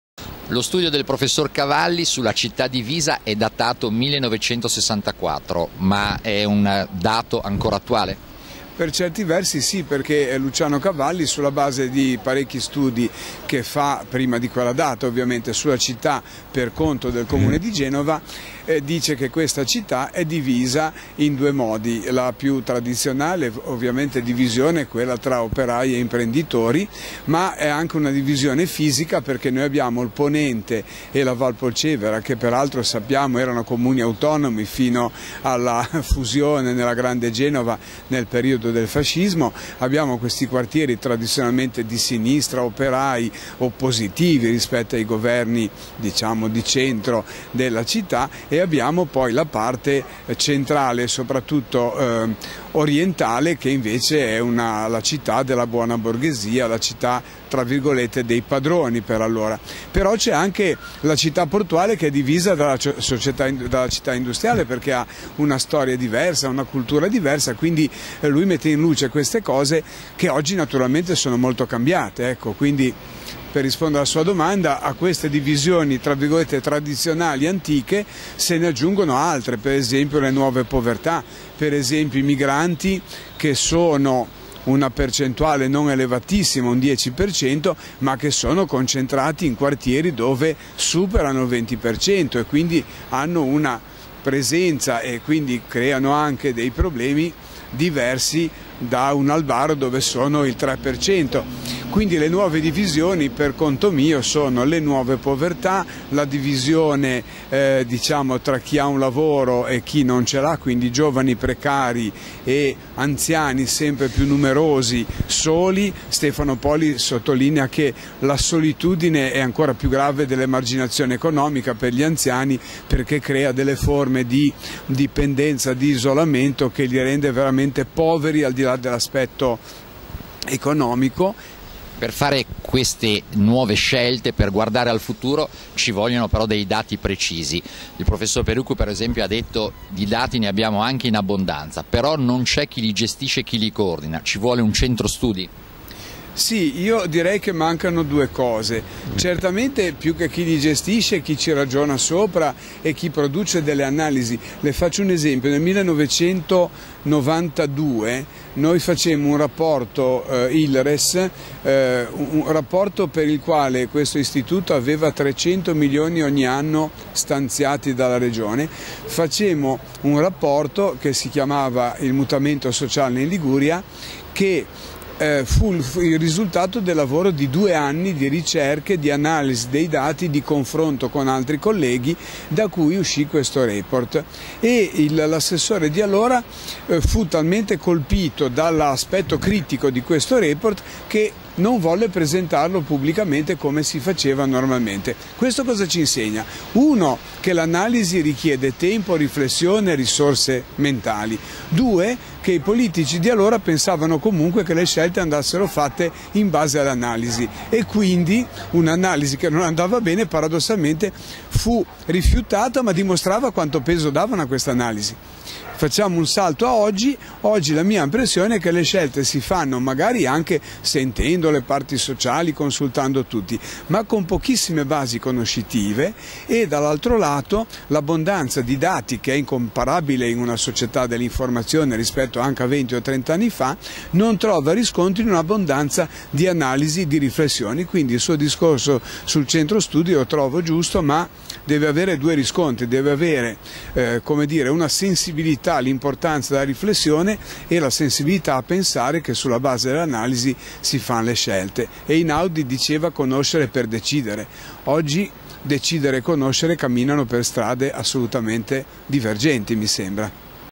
Palumbo " Alle antiche divisioni si aggiungono le nuove povertà, solitudine, precariato "
La città divisa e il futuro dei quartieri tra vecchie e nuove disuguaglianze. Due giorni di confronto all’Università.